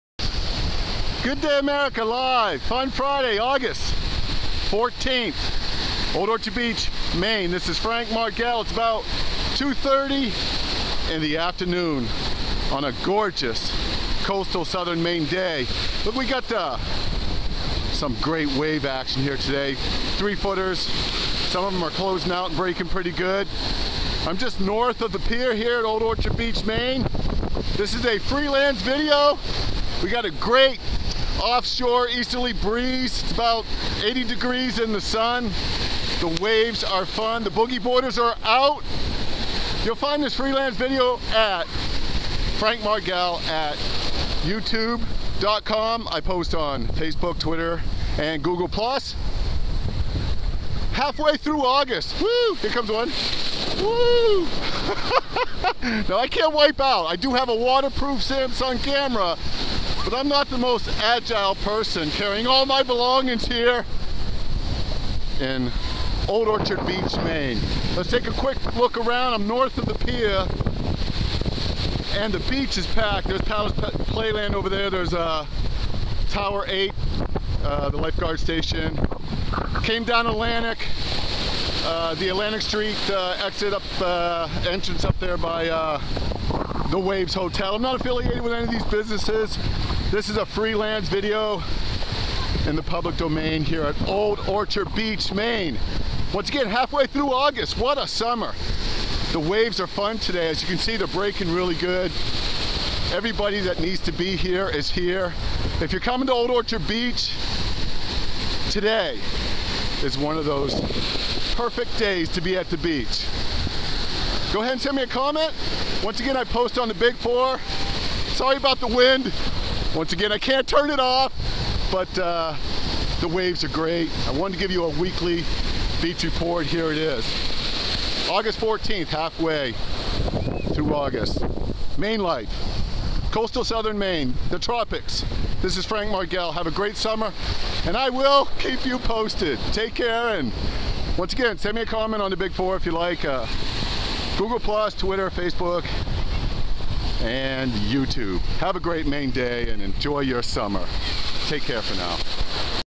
Old Orchard Beach, August Maine Waves, 2015 ~ Frank Margel ~
Spectacular boogie board breakers and cool waves, hot Summer beach life Friday August 14, 2015. Filmed live at Old Orchard Beach, Maine by social media super star and free lance commentator Frank Margel! Enjoy!